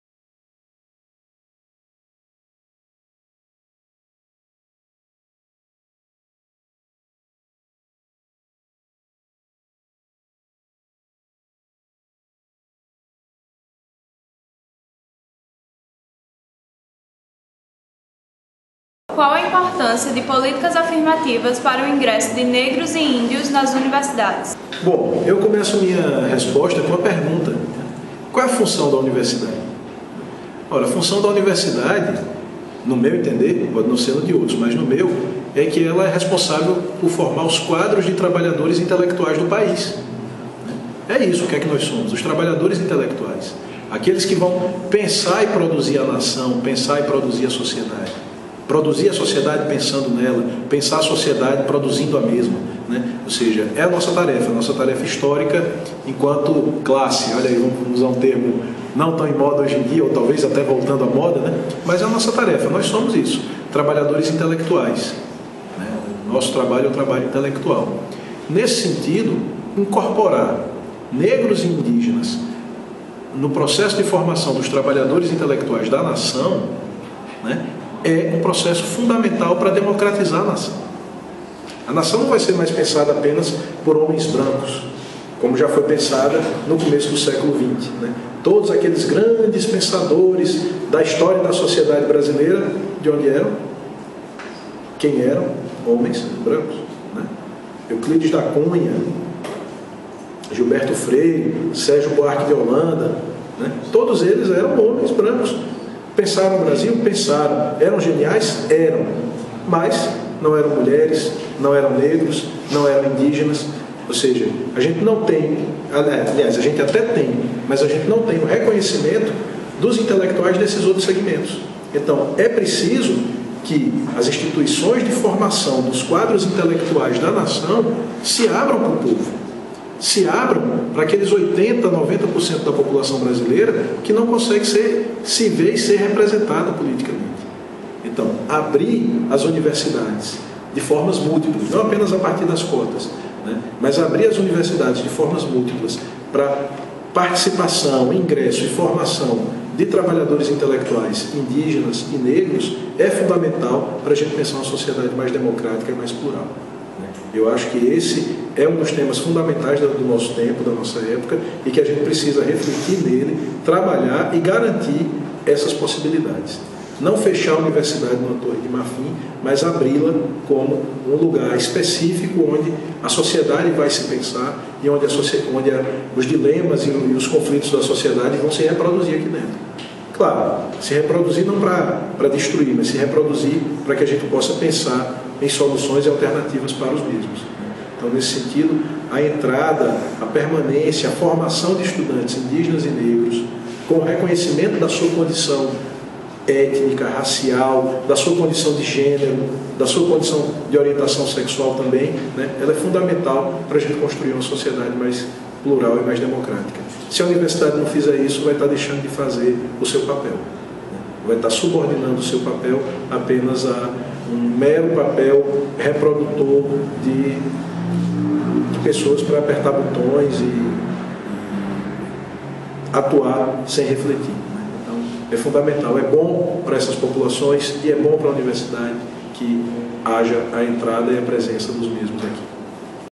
V Colóquio de Diversidade Étnica, com Estevão Palitot - 7
7 – Políticas afirmativas
- O professor destaca a importância de políticas afirmativas para o ingresso de negros e índios nas universidades.
- Sétimo vídeo do V Colóquio sobre Diversidade Étnica, realizado no dia 29 de setembro de 2016 com o professor Estevão Palitot (dos programas de pós-graduação em Antropologia e em Direitos Humanos, Cidadania e Políticas Públicas da UFPB) organizado pelos alunos Carmem Ferreira, Danilo Monteiro, Juliana Souza e Sinara Santos para a disciplina Jornalismo, Cidadania e Direitos Humanos, do curso de Jornalismo da UFPB. Supervisão: Carmélio Reynaldo Ferreira.